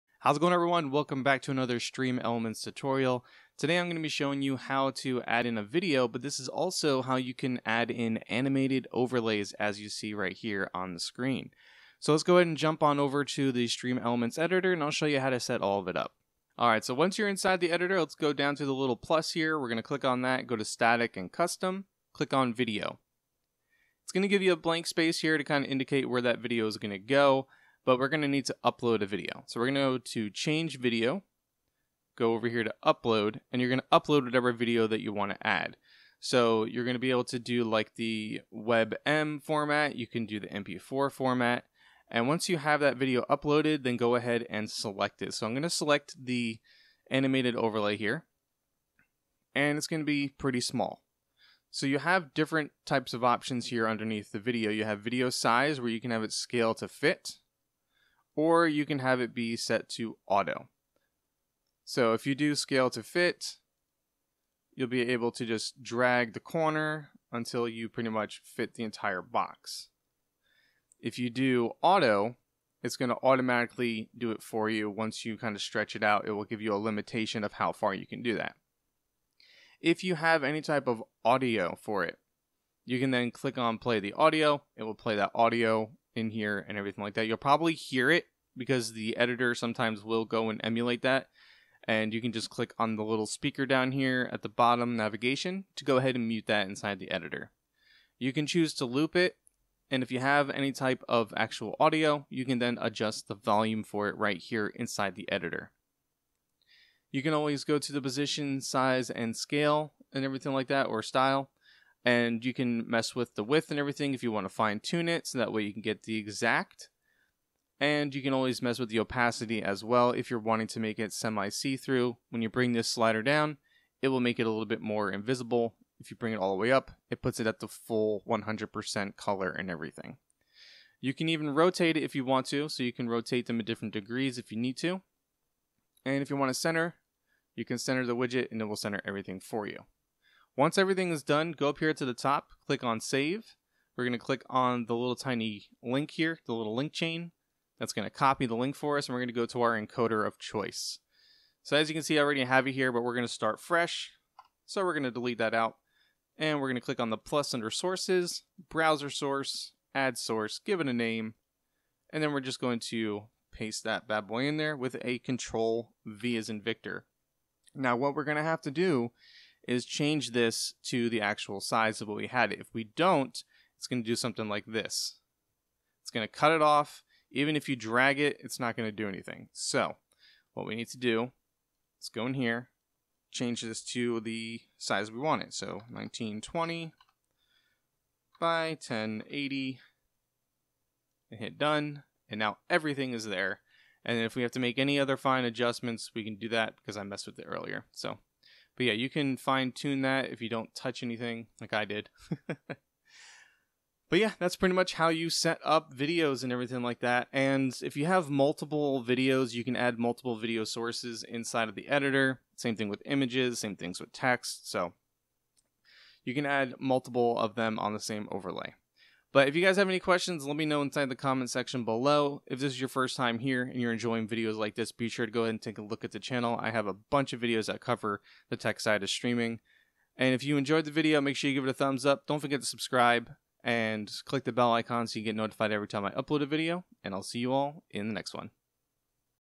Animated overlays In Streamlabs OBS with Streamelements
In this video I show you how to add a video/animated overlay in streamlabs obs using the streamelements video widget.